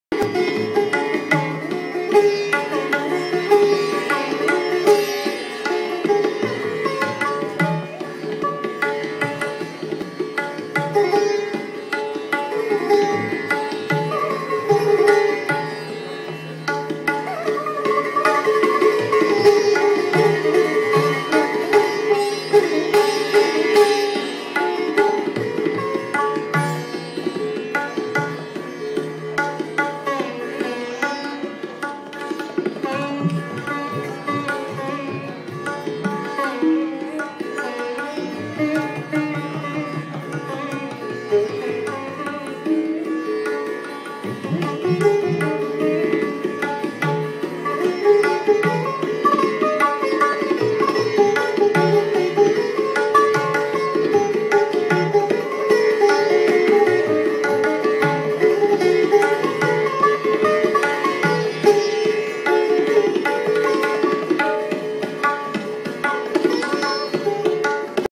Sohini2
Sphoorti benefit concert organized by Anu Anam
Nitin Mitta - Tabla